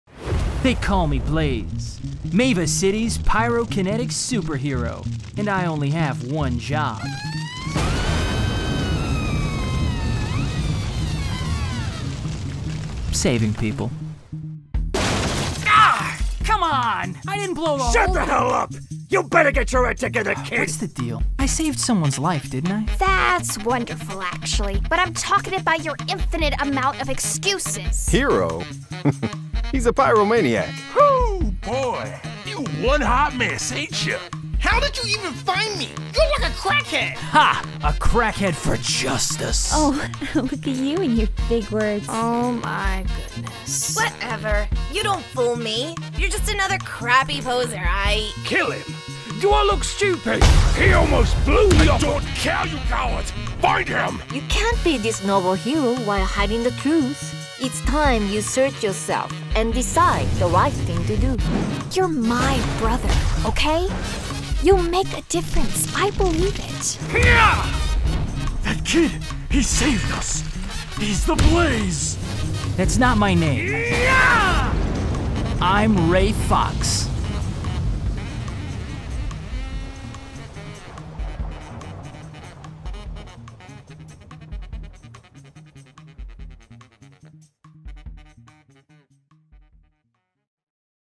Ray Fox - Action/Adventure Webcomic
Credits Below!

Thanks to everyone who have read the comic and supported me in the past couple of years since I've started on it. There's more to come to future, and I don't know when I'll stop so lets enjoy the ride!

If you're new here, you can read the comic Enjoy!

-Credits-

Directed and illustrated by, Myself, Chris (Scotty Artz) Scott. Also the voice of Wasp (00:33)

-Voices Featured-

Elliot Gindi as  Ray Fox, Yak Boxer (00:30), Old man (1:08)

Kevin Couto as Jak (00:17)

Cassie Carey as Liz (00:22)

Chris Monteiro as Morales (00:27)

Serena Paige as Ally (00:39)

Yenni Ann as Summer (00:42)

Alexander Doddy as Halberd (00:48)

Stephen Lester as Liev (00:52)

Yurie Hoyoyon as Asuka (00:54)

-Audio-

Casting/Direction by Elliot Gindi

-Programs used-

Audacity, Adobe Photoshop, and Adobe After Effects